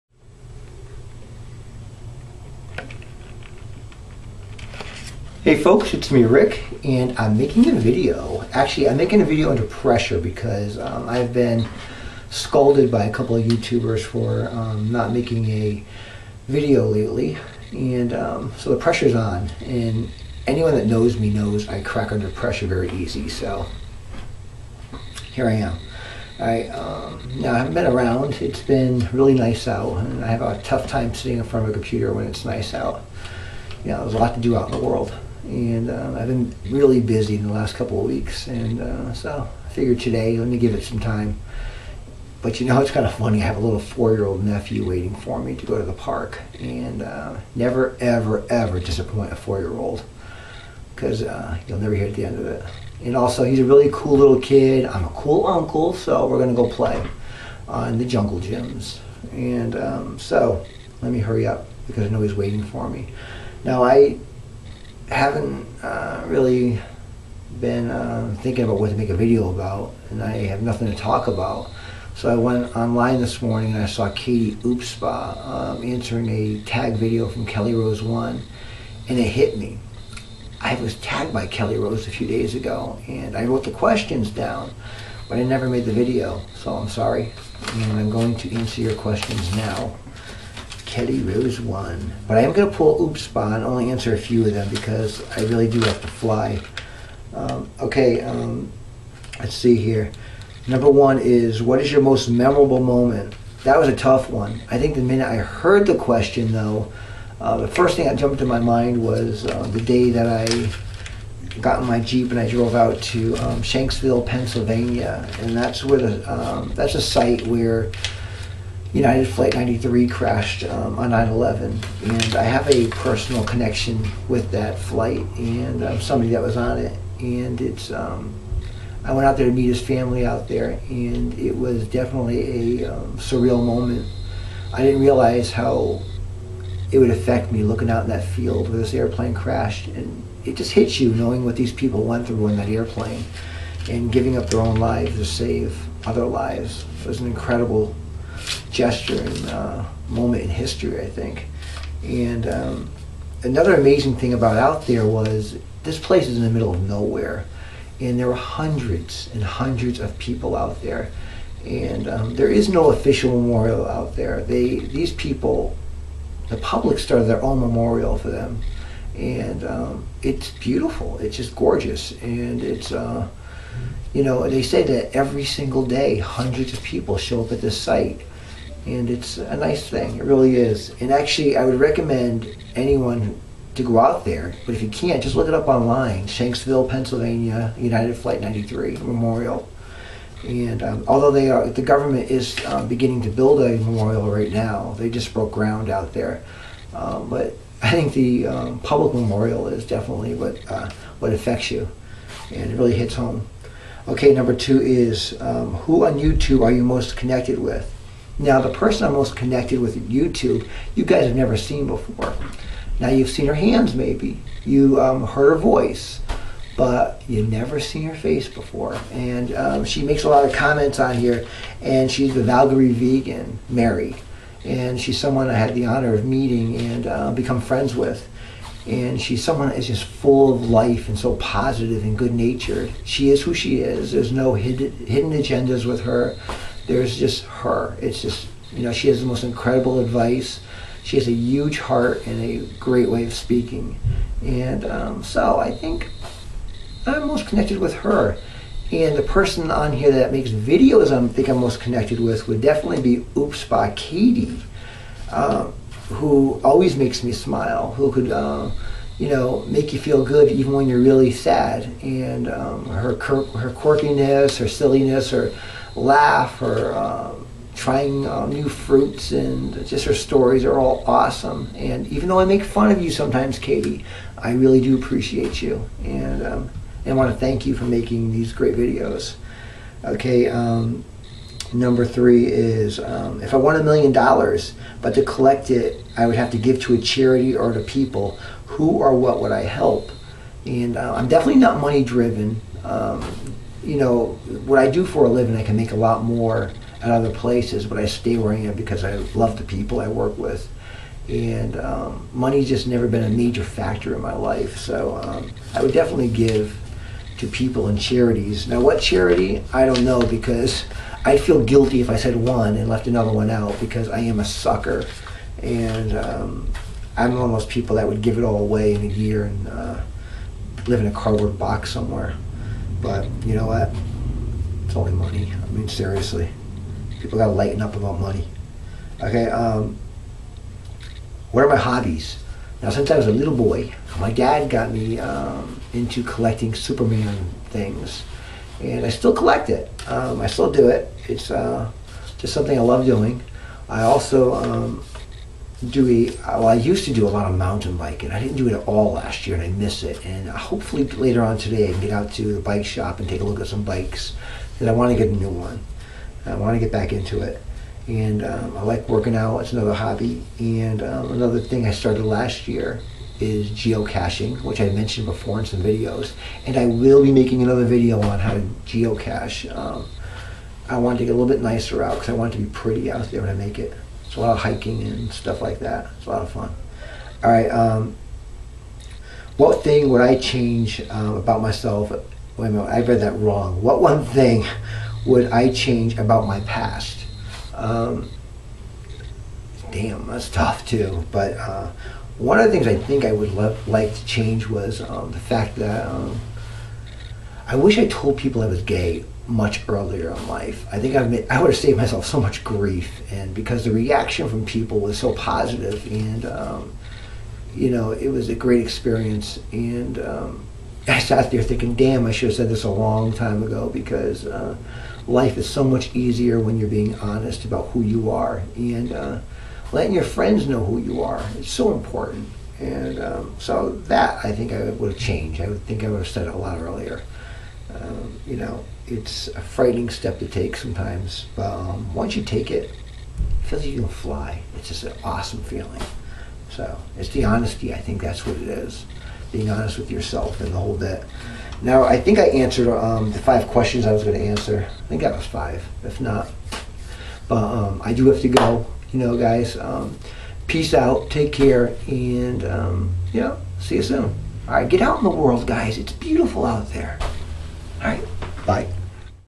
Under Pressure And Gotta Answers Some Questions.
I was tagged by Kellerose1 and I'm under pressure.